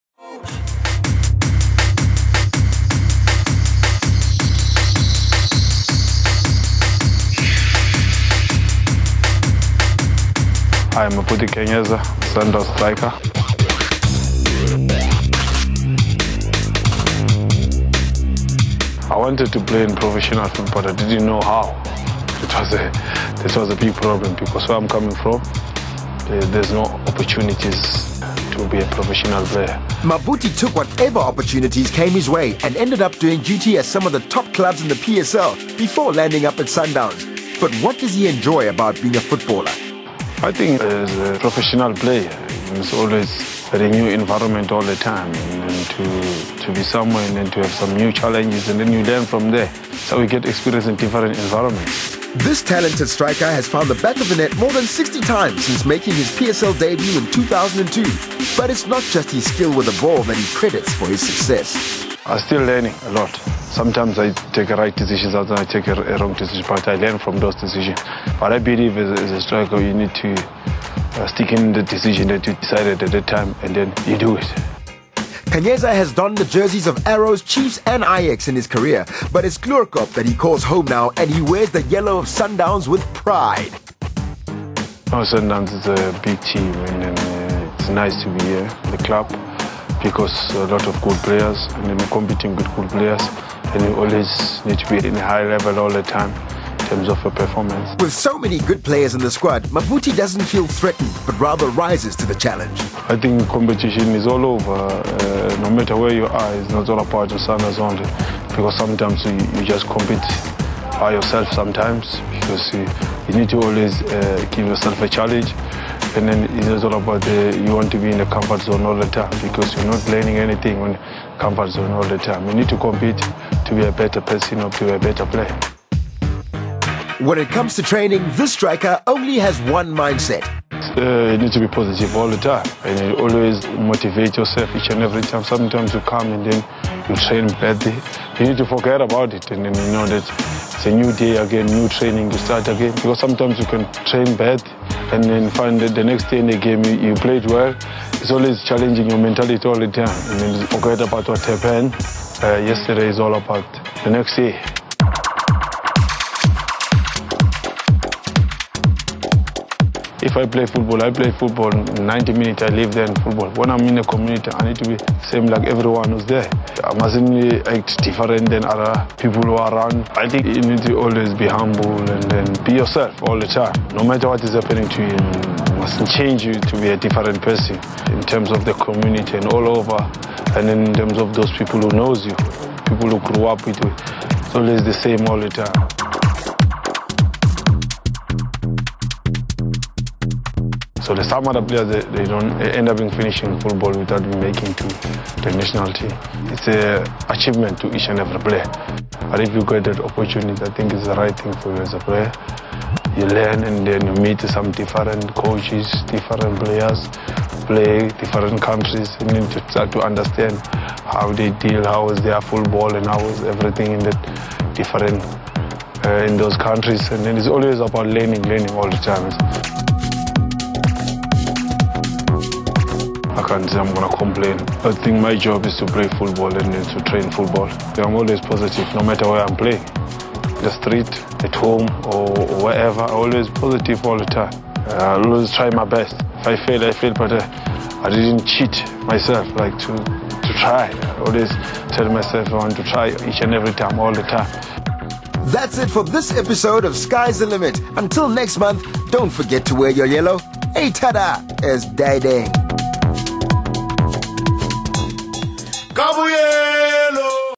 Mabhuti Khenyeza
A sneak peak into our striker, Mabhuti Khenyeza